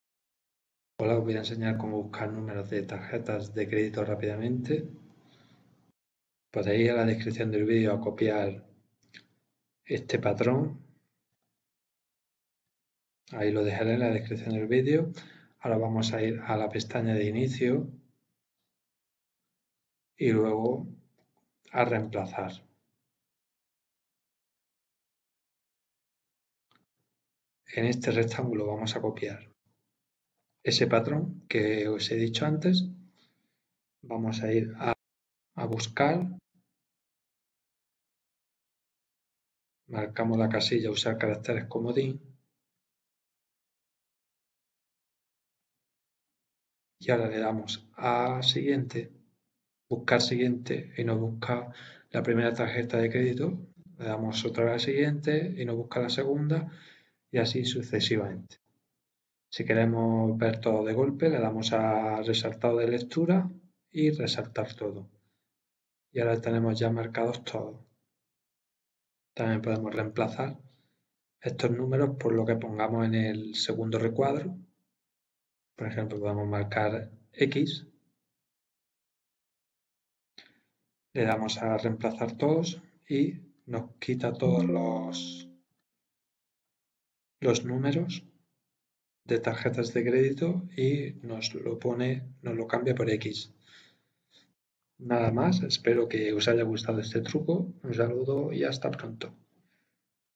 Como buscar y reemplazar números de tarjetas de crédito en Word
Tutorial en vídeo dónde enseño como buscar y reemplazar en Microsoft Word, números de tarjetas de crédito, de una forma muy fácil y rápida.
Se hace usando caracteres comodín con el siguiente patrón

[0-9]{4}[-][0-9]{4}[-][0-9]{4}[-][0-9]{4}

Saludos